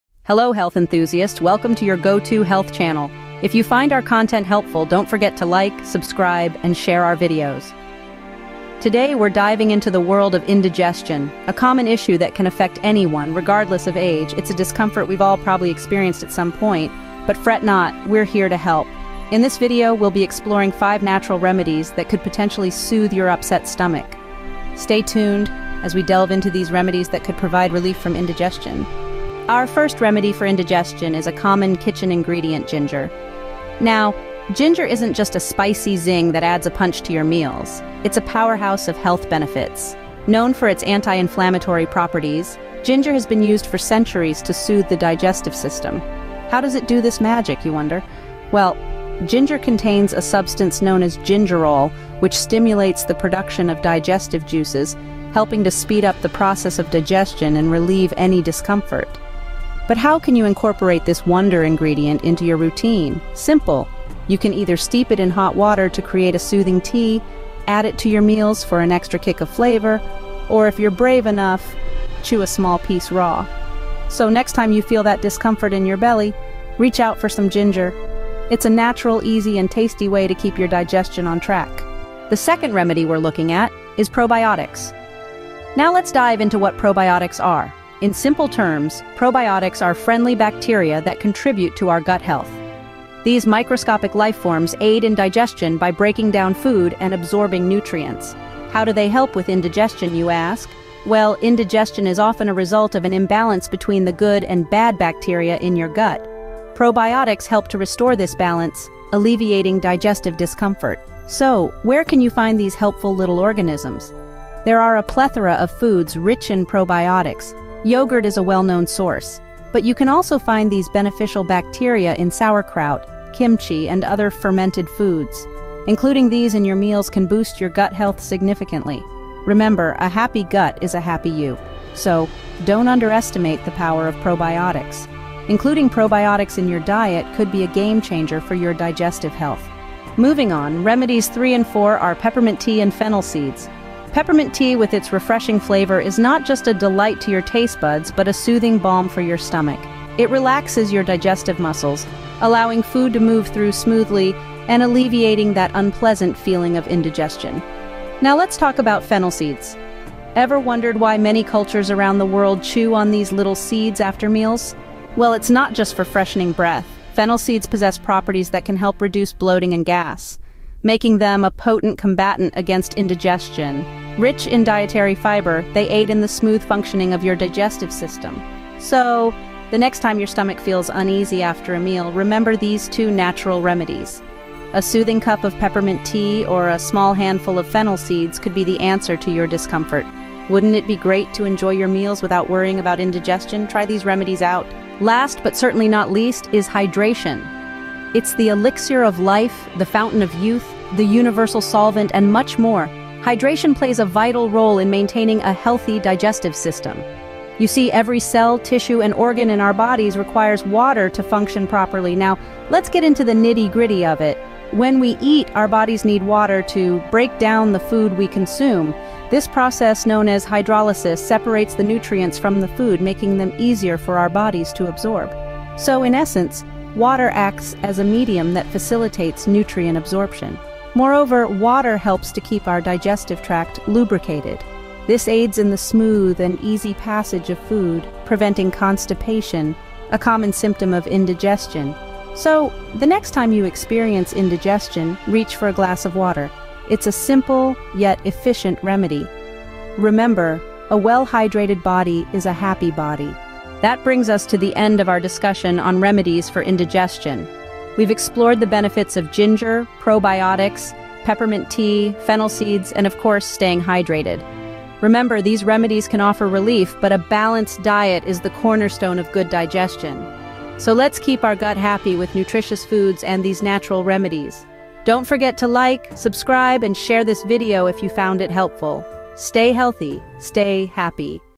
5 QUICK REMEDIES FOR INDIGESTION
If you’ve ever experienced the discomfort of indigestion, you know how important it is to find relief. Today, we’re sharing five effective remedies to help soothe your indigestion. Let’s get started!